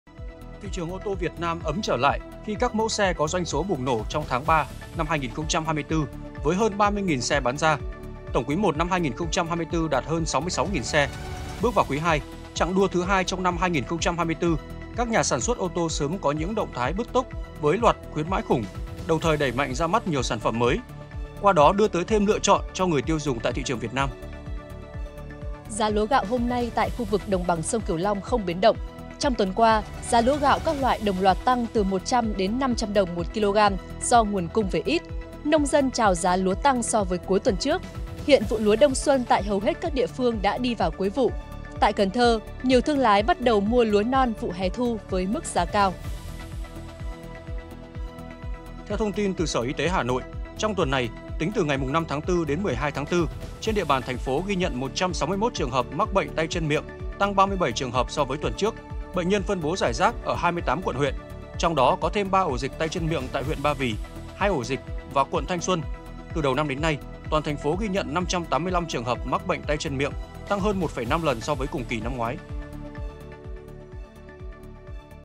Cụm tin vắn Kinh tế - Xã hội ngày 14/04/2024 | Tin tức mới nhất hôm nay
Cụm tin KT – XH:
-       Giá lúa gạo tăng 100 – 500 đồng/kg
- Các hãng ô tô đẩy mạnh khuyến mại
- Mở bán vé tàu hè nhiều khuyến mại
- Hà Nội phát hiện thêm 3 ổ dịch tay chân miệng